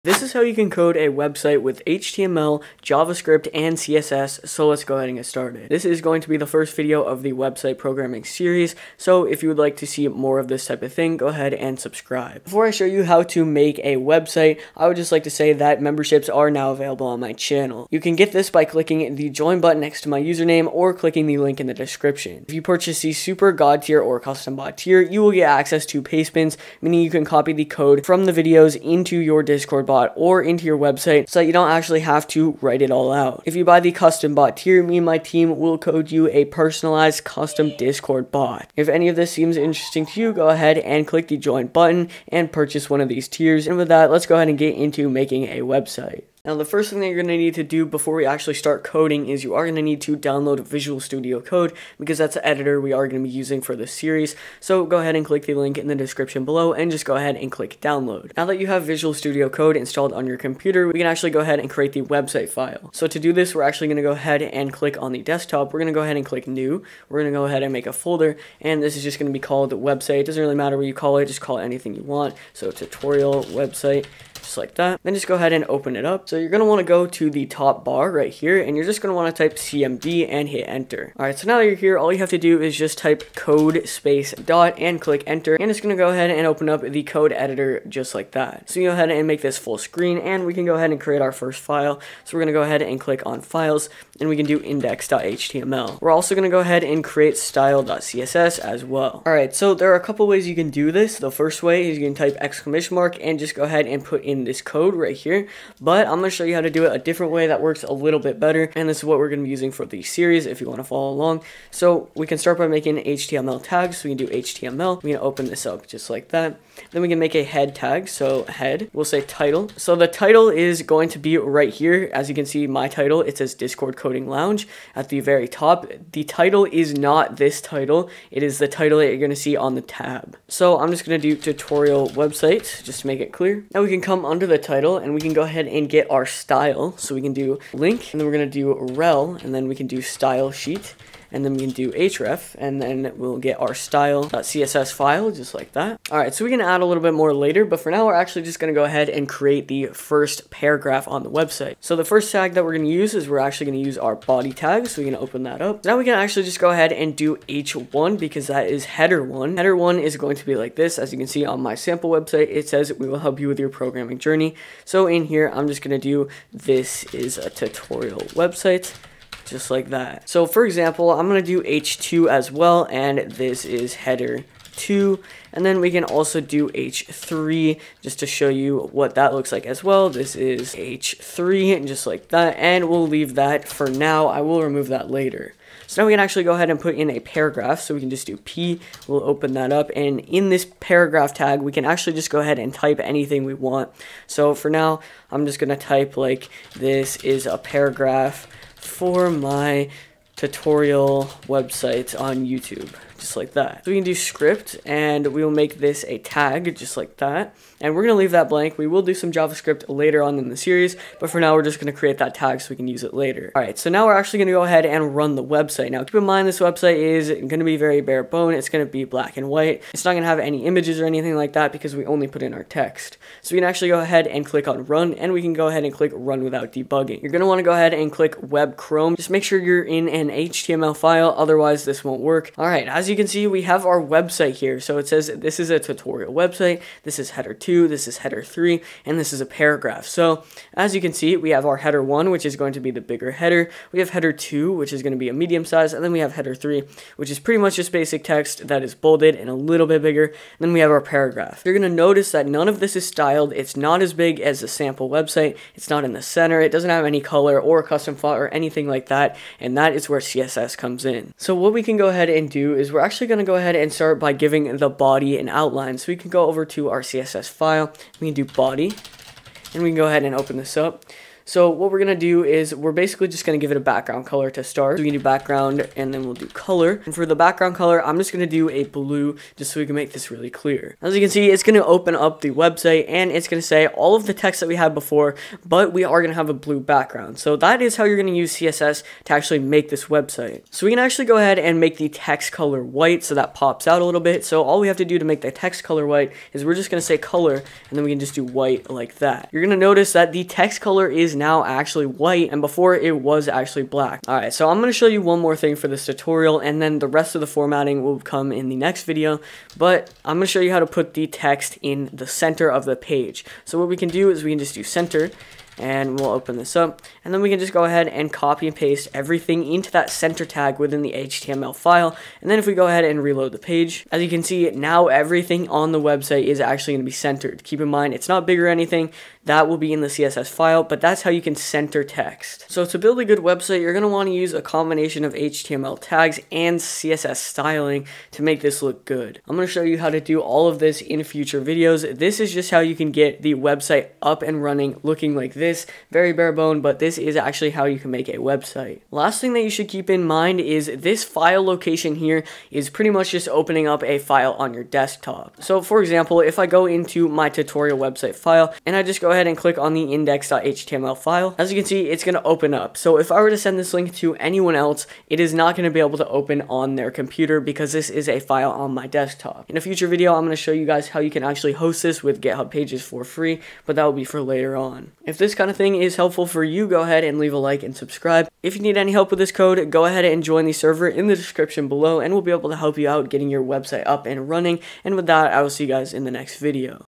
How to code a website using HTML, Javascript & CSS!
This is how you can make a website easily with html, javascript, and css.  This is the start to the series - I will show you how to set up the basic website outline with a bit of css styling!